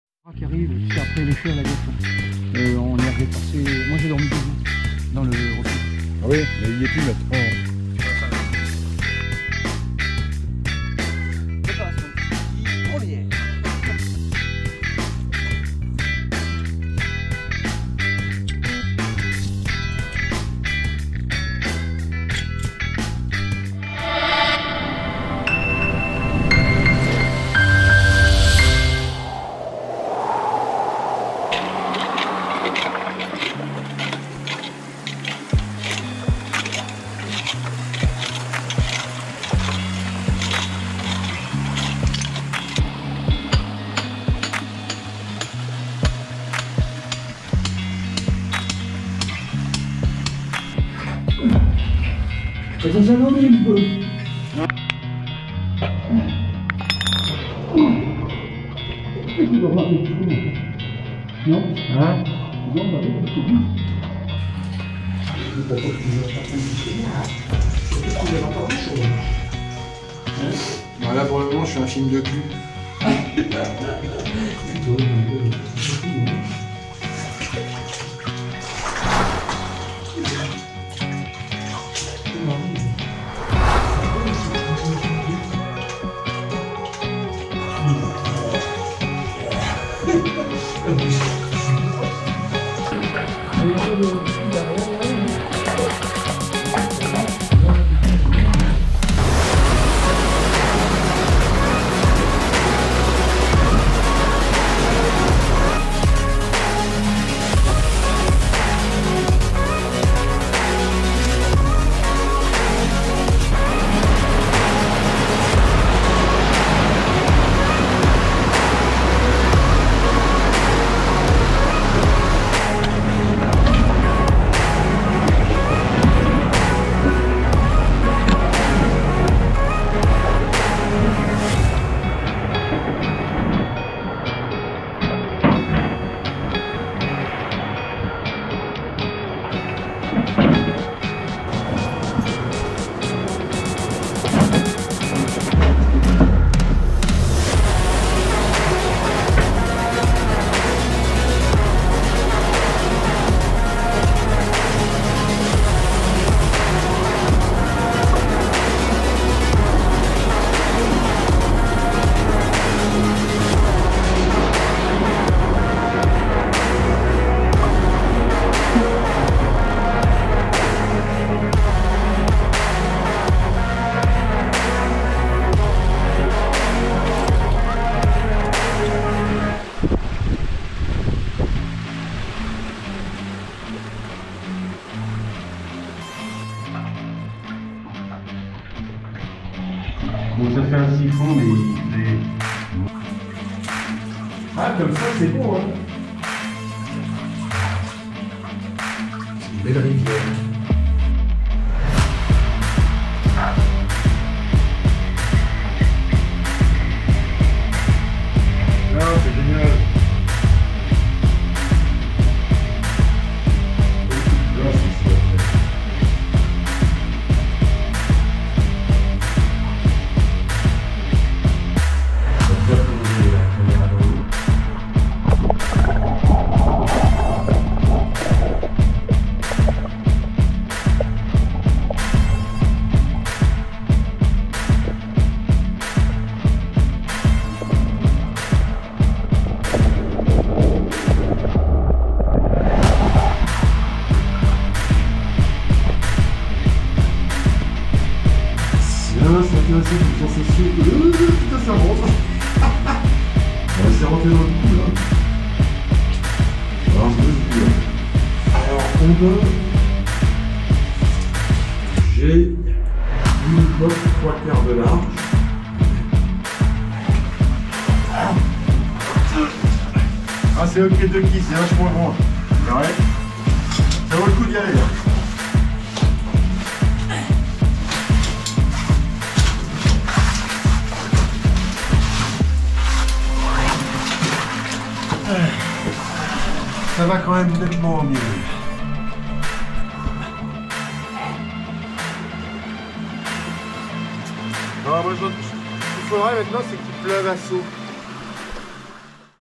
Y a encore de l'eau qui coule ?!?
Après la sortie « pousse cailloux », le temps est venu de tirer le portrait de ce petit trou sympathique. Jipi et Marie sont donc venus en terre d’Aveyronnie, Jipi est équipé de son nouvel outil qui mesure, analyse, enregistre, transmet, et trace instantanément le réseau.

Nous optons pour un équipement à base de néoprène, oh, je sais, il m’est arrivé de me moquer par le passé, mais il faut savoir changer d’avis face à l’adversité.